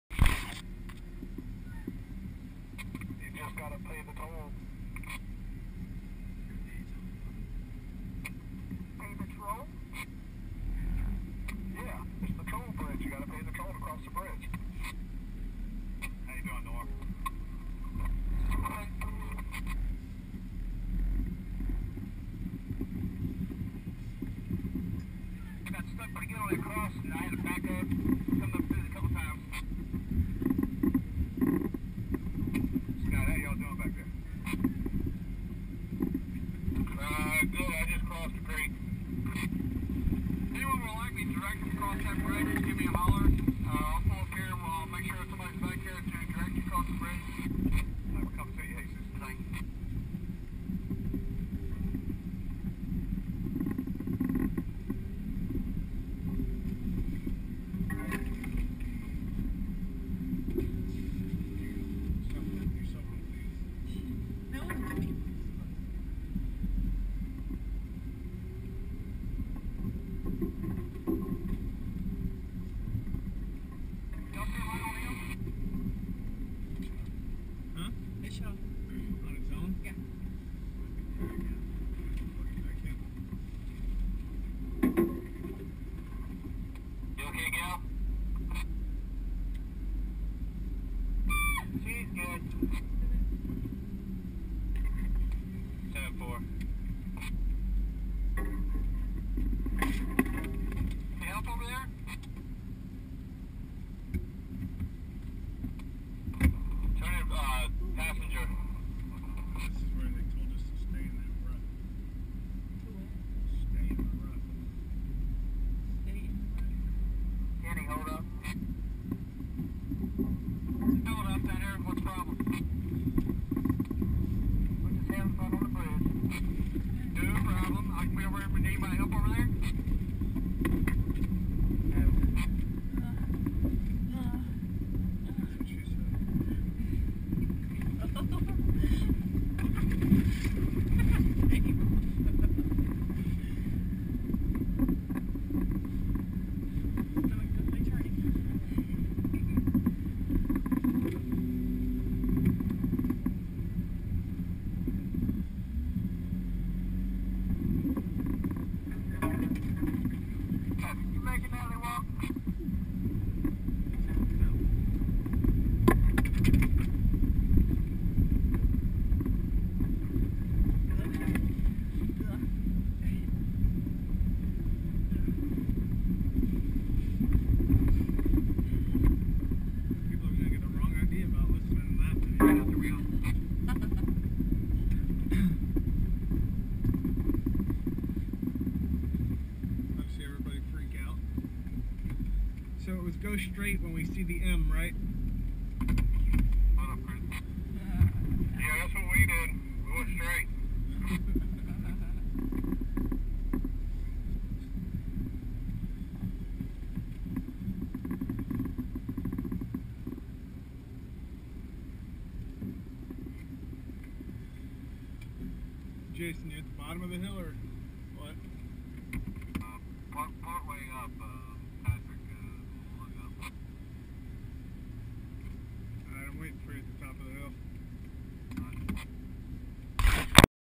Jeep JK Trenched Hill Climb @ Oakridge 2nd Annual Wheeling for Hope - HRJC
Jeep JK Climbs a Trenched out hill @ Oakridge, VA 2nd Annual Wheeling for Hope - HRJC 02APR11

This video was shot with a dash mounted GOPRO Hero camera.